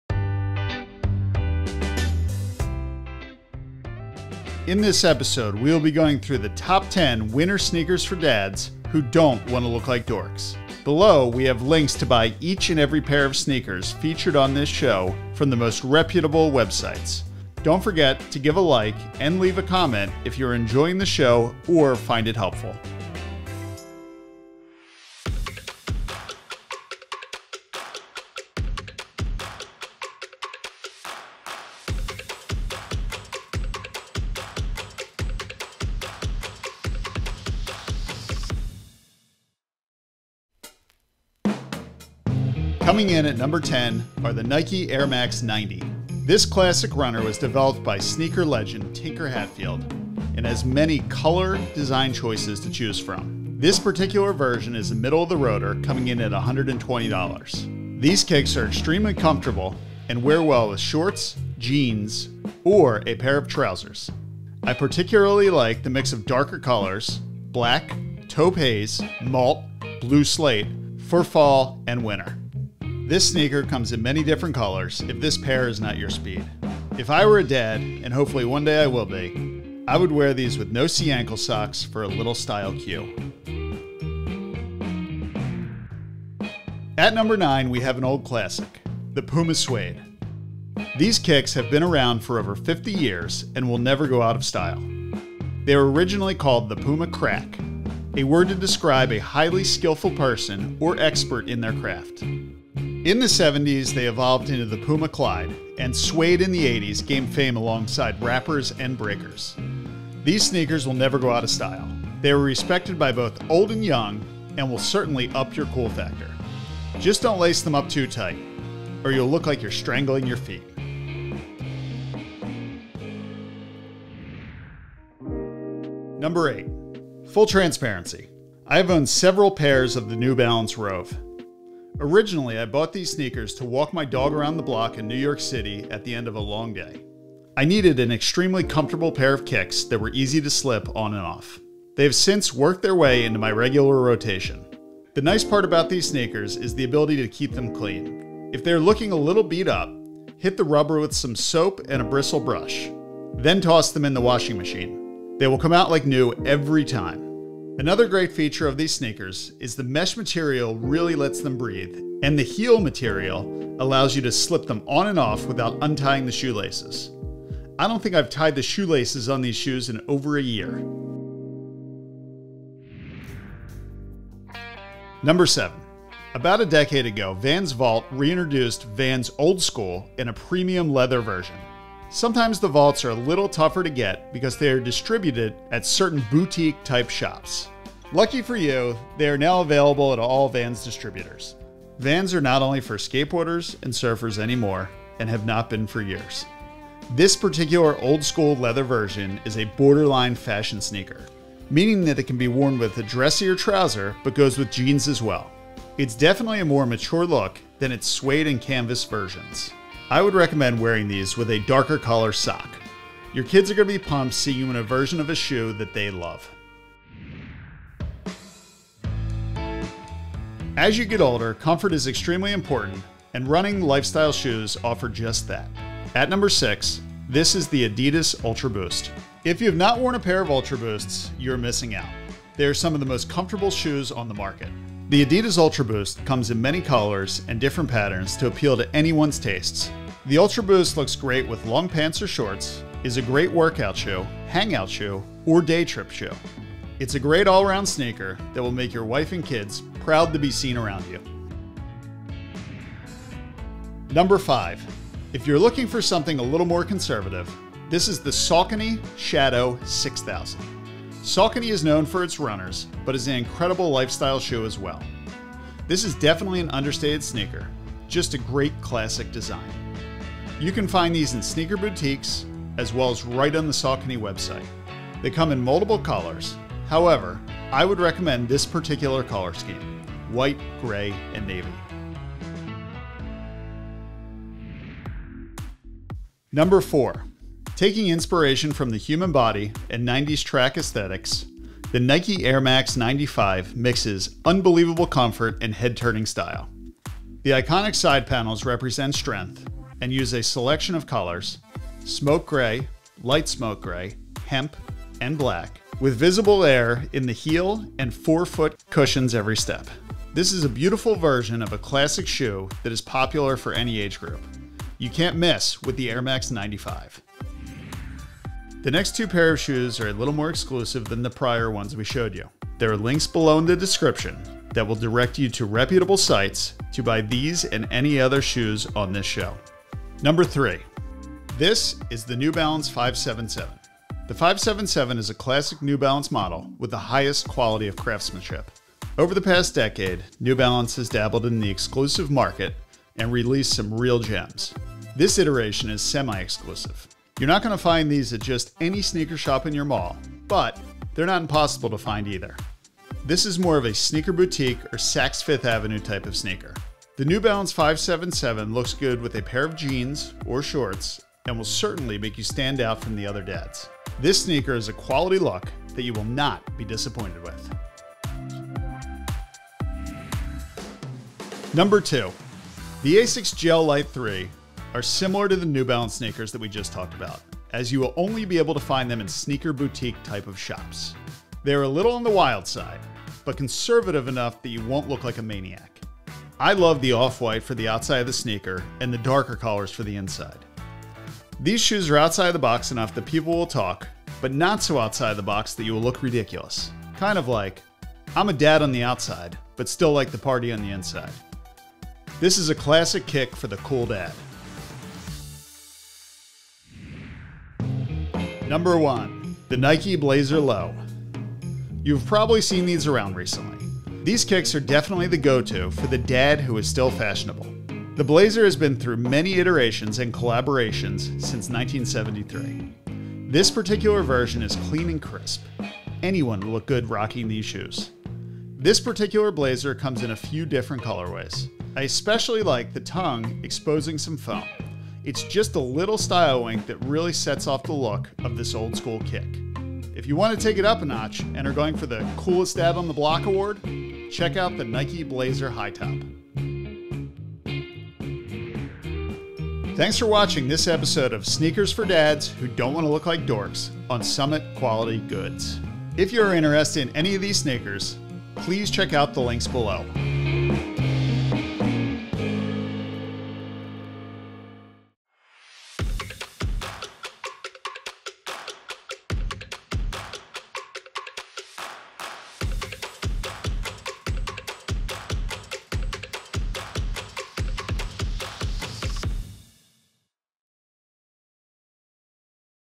Top 10-Sneakers for Dads Who Don't Want to Look Like Dorks
Summit Quality Goods curates the top 10 pairs of sneakers for dads who don't want to look like dorks!  Clickable links below to buy sneakers.

SNEAKERS